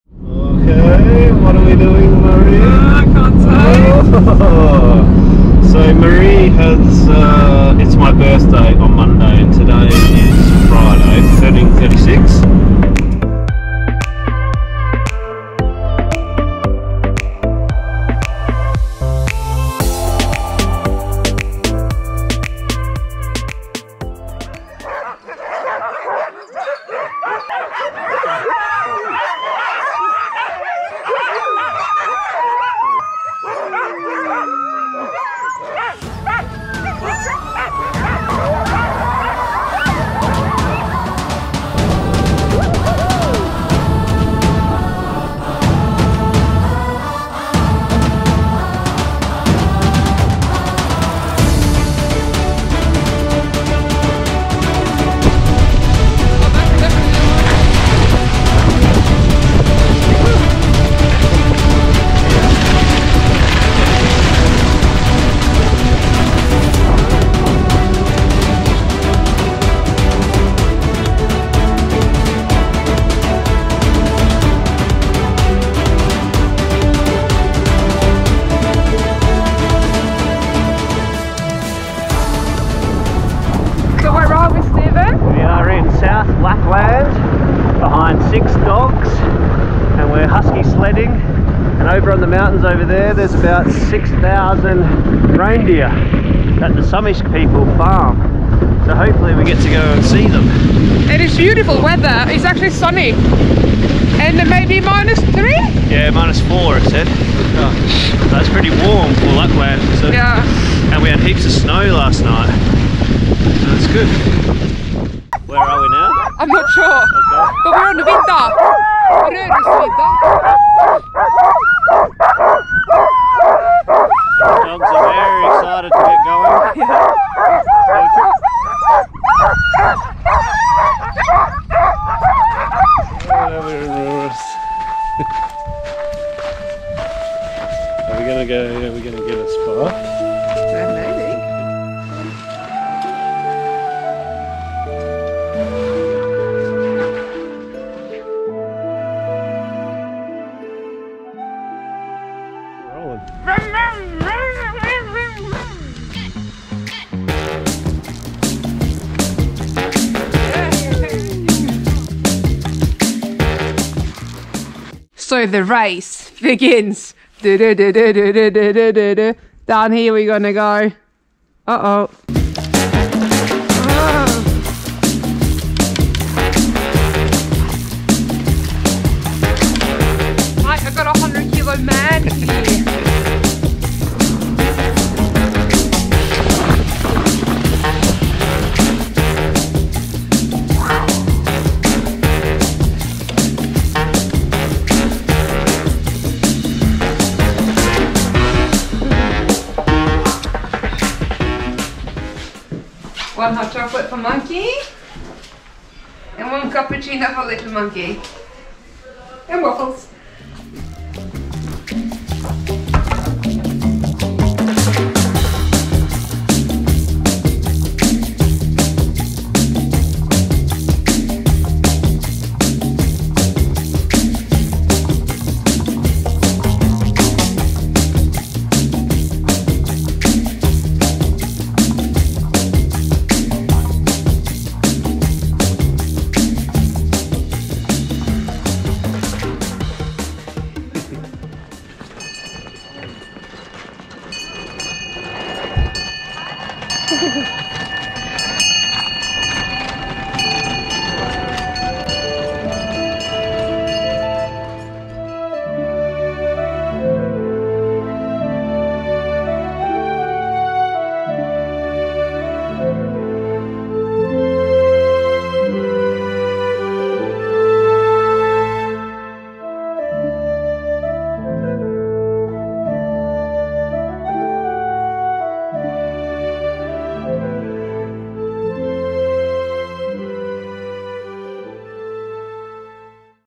Røros husky sledding adventure!
Hi Team,

Mari surprised me for my 36 birthday with a private husky sledding tour in Røros, best present ever!